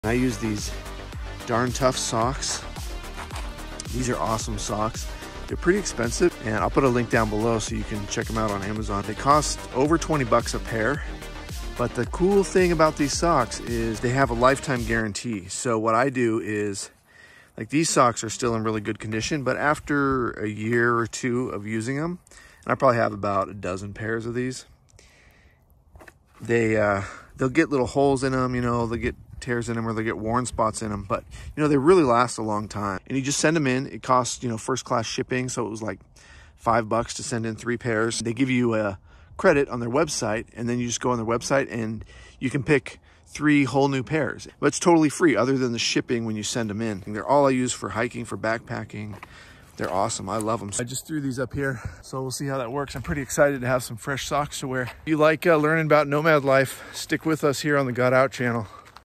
How I Get Free Wool Socks With This Hack 🤑🧦
Lifetime Guaranteed Wool Socks!
Men’s Socks
Women’s Socks
So many colors and styles to choose from!

I love these socks! They are the only socks I wear on my nomadic adventures living in an RV and traveling the Western United States with my dogs.